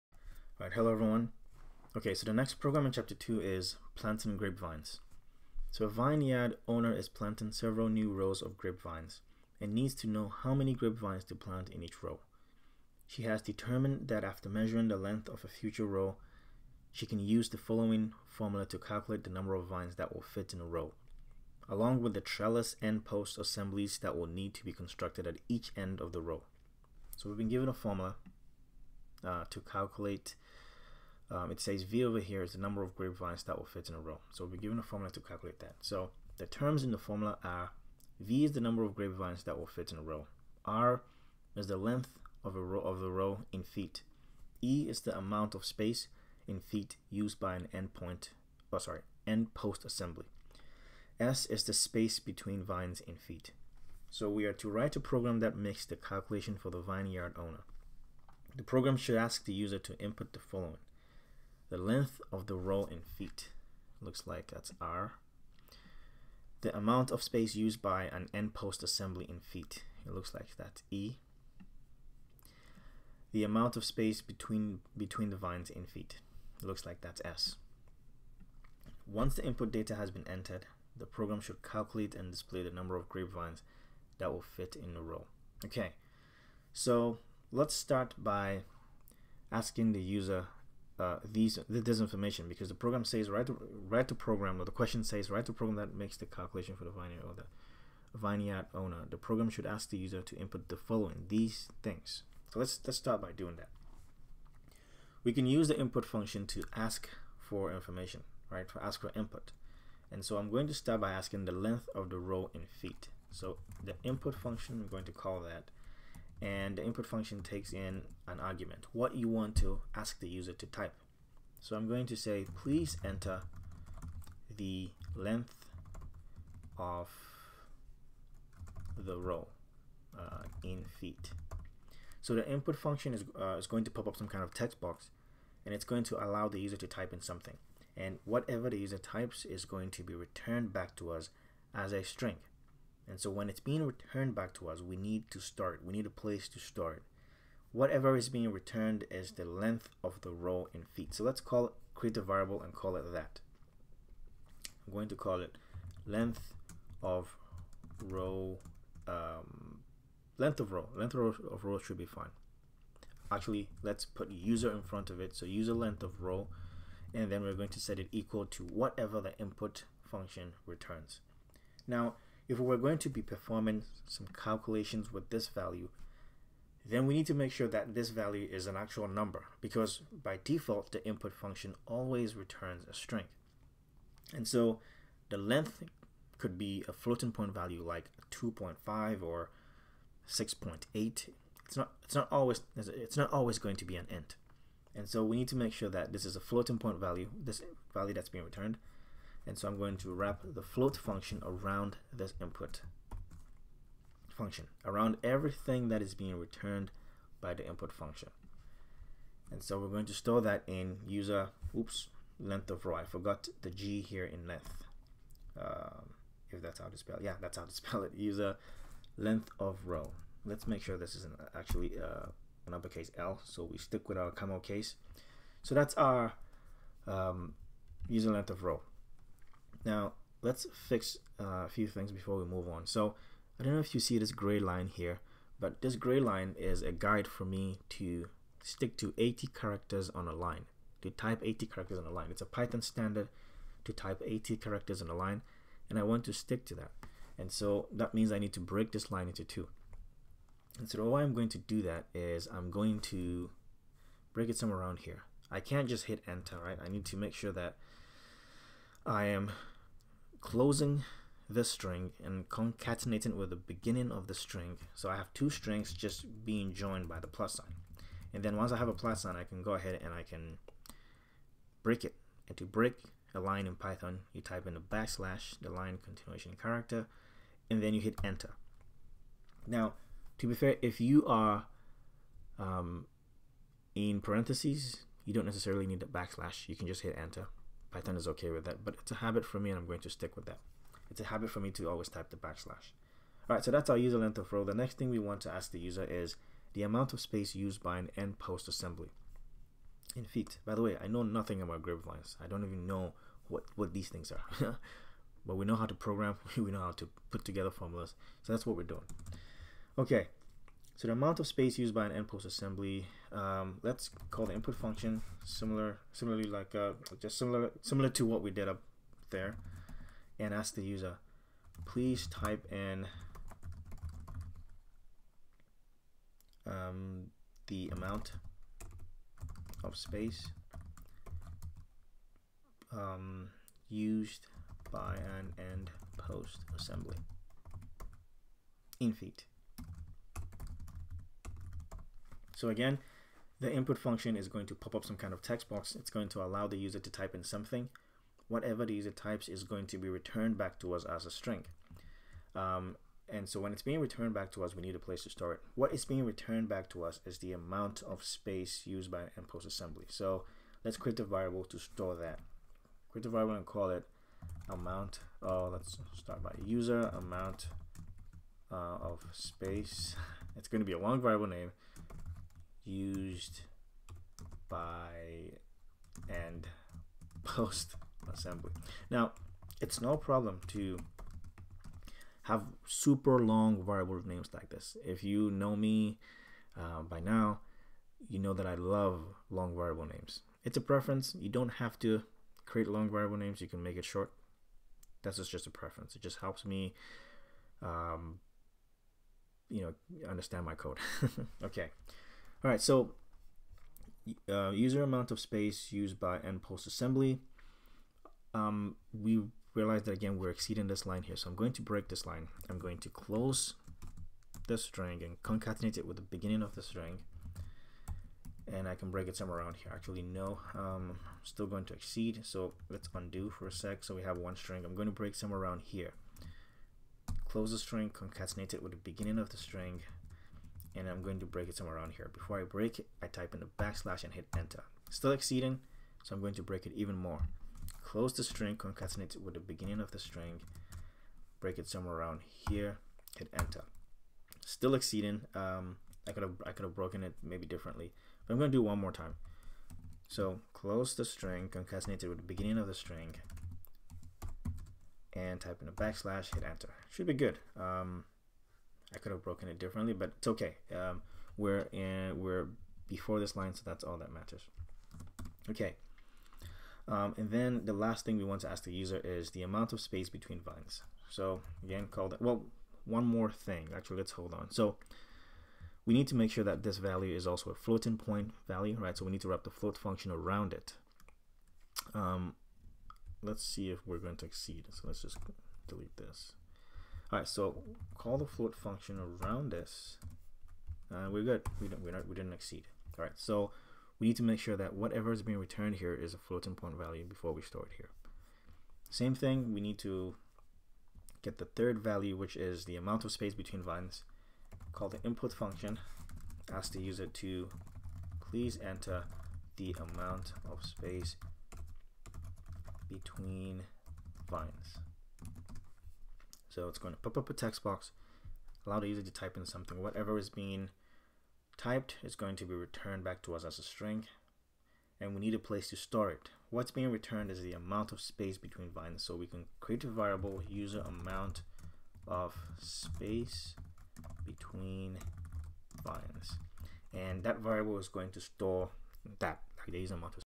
2.13. (Part 1) Planting Grapevines - Python
Starting out with Python, Fourth Edition, Tony Gaddis

Chapter 2
Programming Challenges

13. Planting Grapevines

A vineyard owner is planting several new rows of grapevines, and needs to
know how many grapevines to plant in each row. She has determined that
after measuring the length of a future row, she can use the following
formula to calculate the number of vines that will fit in the row,
along with the trellis end-post assemblies that will need to be constructed
at each end of the row:

V = ( R - 2E ) / S

The terms in the formula are:

V is the number of grapevines that will fit in the row.
R is the length of the row, in feet.
E is the amount of space, in feet, used by an end-post assembly.
S is the space between vines, in feet.

Write a program that makes the calculation for the vineyard owner.
The program should ask the user to input the following:

• The length of the row, in feet
• The amount of space used by an end-post assembly, in feet
• The amount of space between the vines, in feet

Once the input data has been entered, the program should calculate and
display the number of grapevines that will fit in the row.